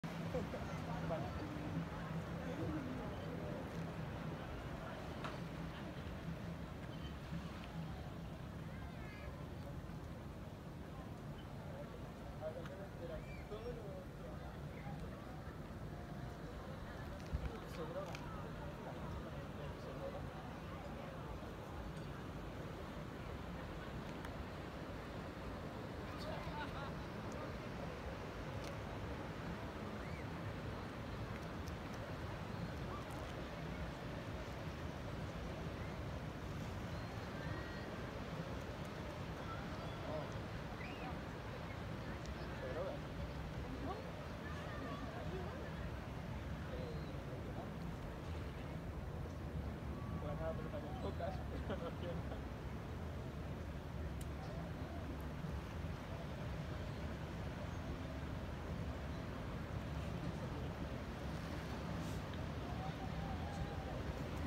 Sunset Sitges 09/16/23 19:47
iPhoneが壊れた。写真が変で面白い。My iPhone broke. The photos are weird and interesting.
Sitges News showcases the best of Sitges.Sitges News はSitgesの良いところを紹介します。